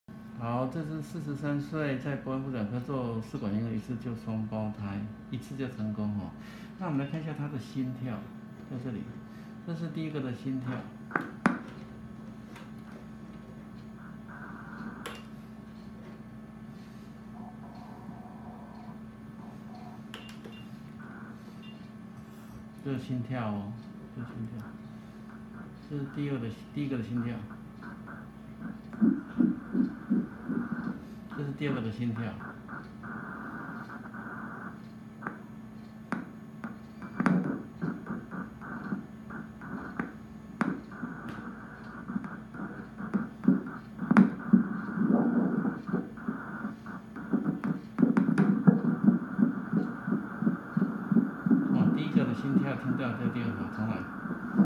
43
參加FastTrack  ​ivf​   快速​ 試管嬰兒研究可獲的學術減免手術費

博元婦產科進行前瞻性學術研究:做試管嬰兒可能一次就成功懷孕​嗎?
我們想回答這問題答案是什麼?

如果妳參加這個研究可獲的學術減免手術費更令人期待的:有機會試管嬰兒一次成功
博元婦產科 ​電話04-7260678 分機123

​博元婦產科​ 地址:彰化市中正路一段392號(彰化火車站旁邊)
​做試管嬰兒可能一次就成功懷孕​嗎?答案似乎接近可能

試管嬰兒如果要成功以下三個條件一個也不能少:並且植入兩個胚胎以上

1.胚胎的全部染色體經過晶片檢查24對染色體是正常的

2.子宮內膜有三條線超過厚度0.8公分以上

3.胚胎在子宮頂距離是1.5-2公分合併超音波導引植入胚胎

如果以上3個都完成並且植入兩個胚胎以上

應可達成試管嬰兒一次成功的目標

當然要完成這3個條件不是這麼容易

尤其第一個條件要有全部24染色體經由基因晶片ACGH檢查或NGS檢查染色體都正常

需要相當多的卵子數目進行(1)受精(2)胚胎培養囊胚孵化出來(3)胚胎切片

一般的統計發現胚胎經過染色體篩檢只有不到一半是染色體正常

所以要植入兩個24染色體正常的胚胎是相當不容易達到的

這也是為什麼試管嬰兒有些人體質不容易一次成功的原因

如果你辦到以上3件事情試管嬰兒要一次成功其實不難